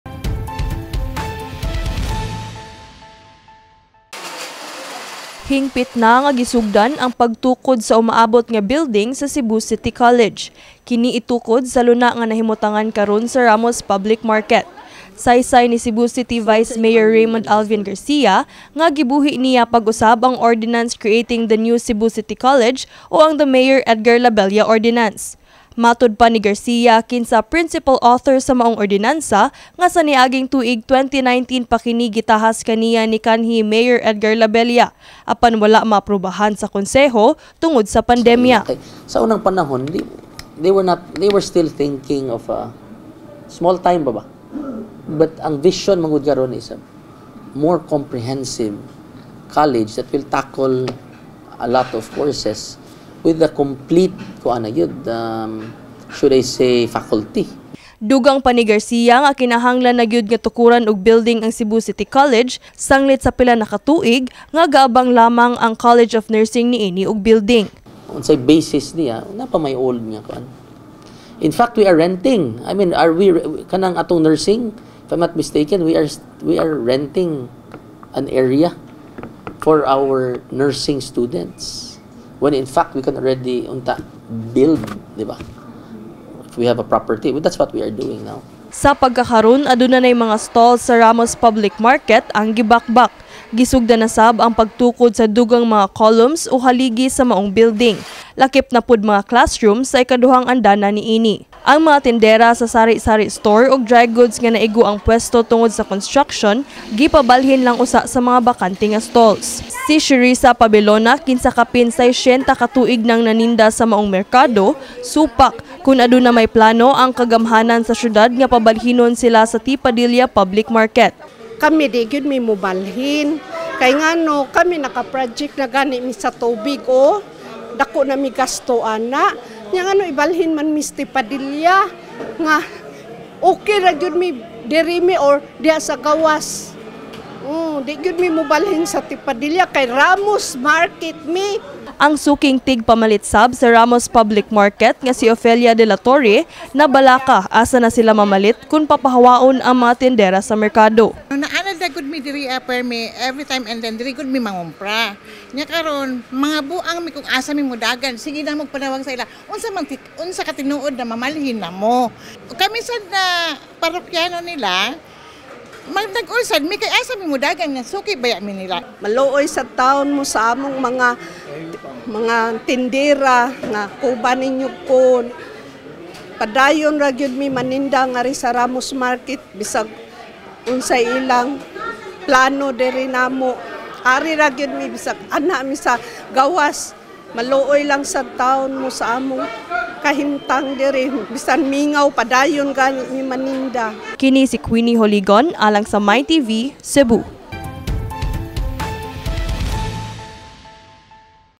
𝗠𝘆𝗧𝗩 𝗡𝗲𝘄𝘀 𝗨𝗽𝗱𝗮𝘁𝗲: 𝗚𝗶𝘀𝘂𝗴𝗱𝗮𝗻 𝗻𝗮 𝗸𝗮𝗿𝗼𝗻 𝗮𝗻𝗴 𝗽𝗮𝗴𝘁𝗿𝗮𝗯𝗮𝗵𝗼 𝘀𝗮 𝗯𝘂𝗶𝗹𝗱𝗶𝗻𝗴 𝘀𝗮 𝗖𝗲𝗯𝘂 𝗖𝗶𝘁𝘆 𝗖𝗼𝗹𝗹𝗲𝗴𝗲 𝗮𝗽𝗮𝗻 𝗺𝗶𝗽𝗮𝗱𝗮𝘆𝗮𝗴
𝗠𝘆𝗧𝗩 𝗡𝗲𝘄𝘀 𝗨𝗽𝗱𝗮𝘁𝗲: 𝗚𝗶𝘀𝘂𝗴𝗱𝗮𝗻 𝗻𝗮 𝗸𝗮𝗿𝗼𝗻 𝗮𝗻𝗴 𝗽𝗮𝗴𝘁𝗿𝗮𝗯𝗮𝗵𝗼 𝘀𝗮 𝗯𝘂𝗶𝗹𝗱𝗶𝗻𝗴 𝘀𝗮 𝗖𝗲𝗯𝘂 𝗖𝗶𝘁𝘆 𝗖𝗼𝗹𝗹𝗲𝗴𝗲 𝗮𝗽𝗮𝗻 𝗺𝗶𝗽𝗮𝗱𝗮𝘆𝗮𝗴 𝘀𝗮 𝗽𝗮𝗴𝘀𝘂𝗽𝗮𝗸 𝗮𝗻𝗴 𝗺𝗴𝗮 𝗺𝗮𝗻𝗶𝗻𝗱𝗮𝗵𝗮𝘆 𝘀𝗮 𝗥𝗮𝗺𝗼𝘀 𝗣𝘂𝗯𝗹𝗶𝗰 𝗠𝗮𝗿𝗸𝗲𝘁 𝘁𝘂𝗻𝗴𝗼𝗱 𝗮𝗽𝗲𝗸𝘁𝗮𝗱𝗼 𝘀𝗶𝗹𝗮 𝘀𝗮 𝗺𝗮𝗼𝗻𝗴 𝗽𝗿𝗼𝘆𝗲𝗸𝘁𝗼.

Gitakdang mapabarog ang building sa nahimutang sa maong merkado.

Ania si Queenie Joligon sa report.